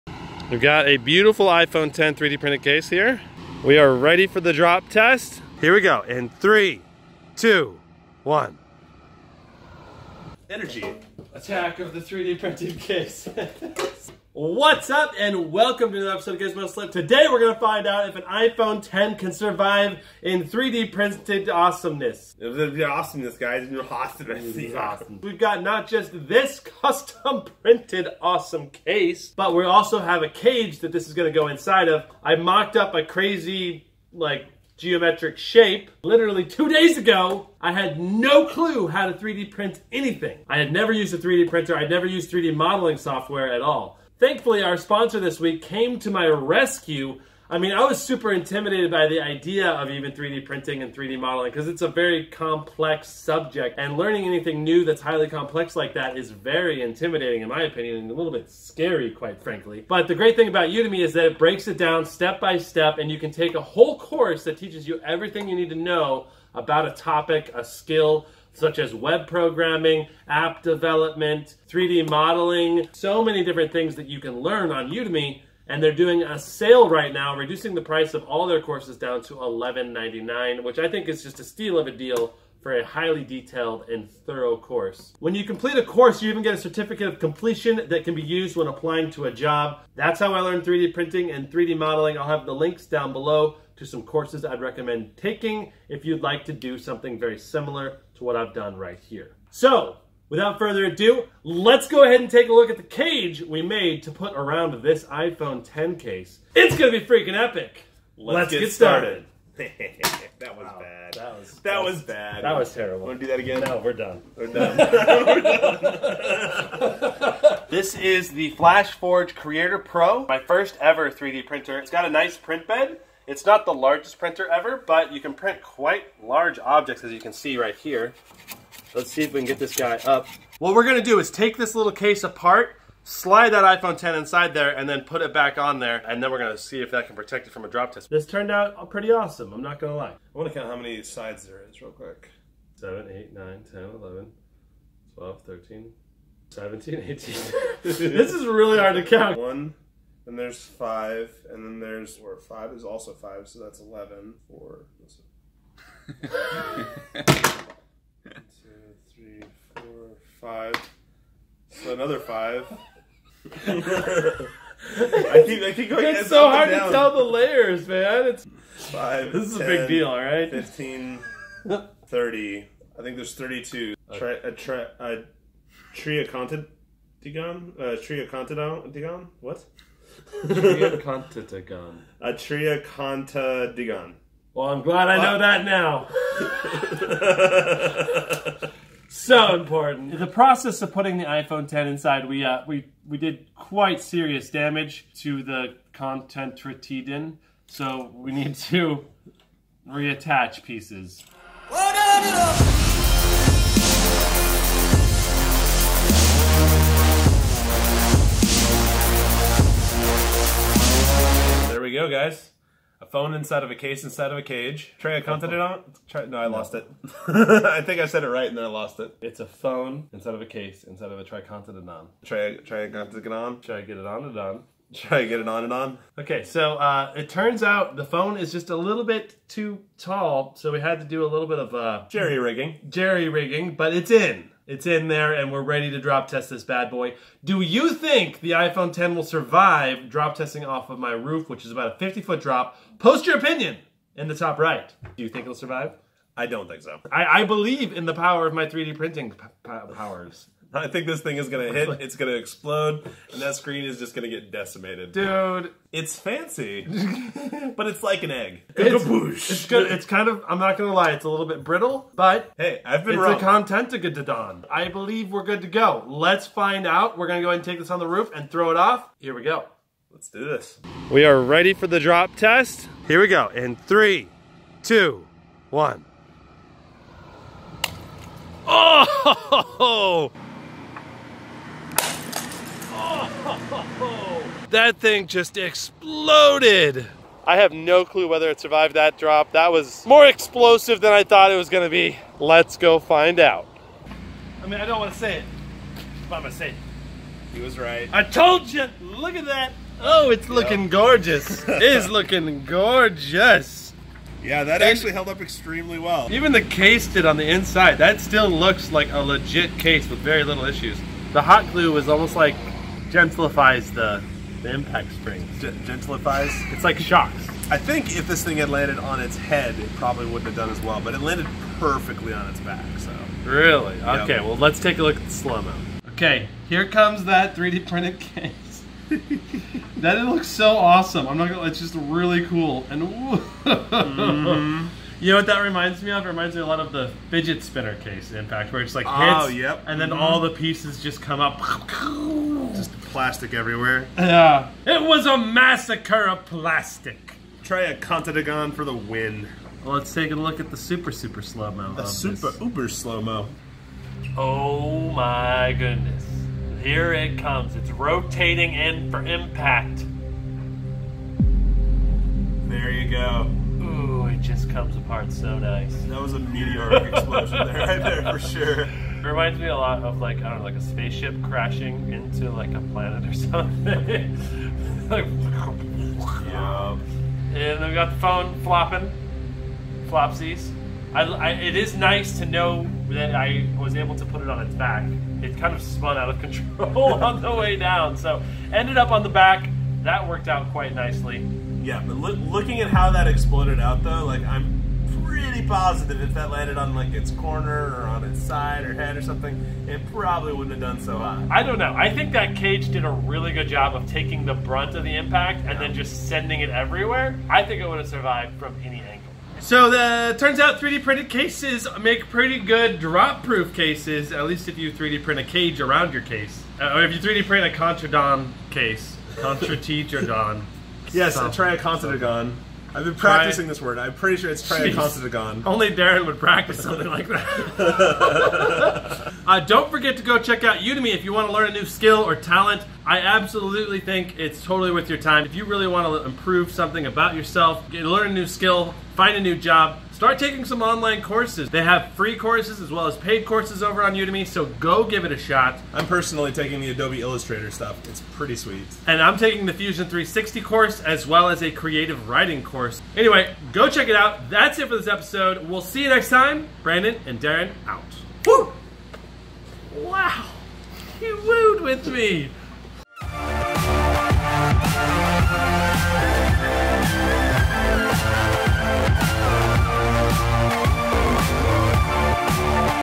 Can iPhone X Survive 50 FT Drop Test in 3D Printed Case?!
I 3D printed a wacky iPhone X case with a ridiculous 32 sided triacontadigon cage around it! 3D priting is difficult to learn, and I had to rebuild the case several times to get it to come out perfectly. But now the question is, can it protect an iPhone X from a 50 FT drop test! Let's find out!